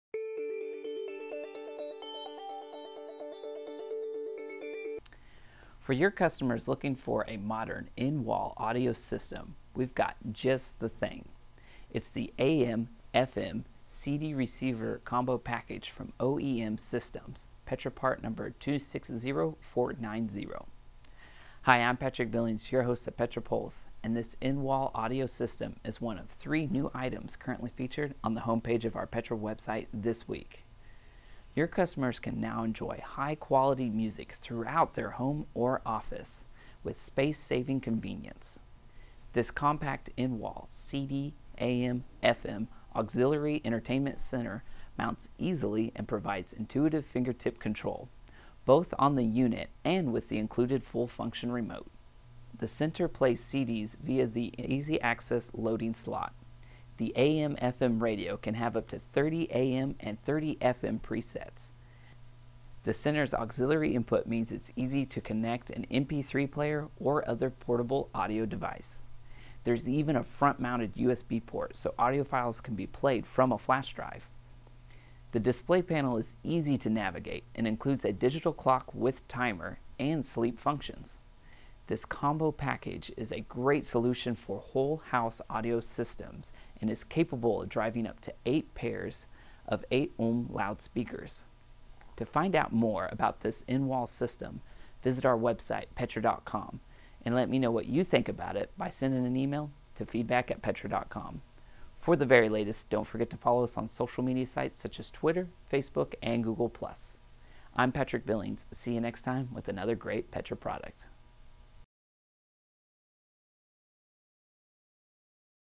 OEM Systems AM/FM/CD Receiver Combo Package
For your customers looking for a modern in-wall audio system, we've got just the thing. It's the AM/FM/CD Receiver Combo Package from OEM Systems, Petra part number 260490.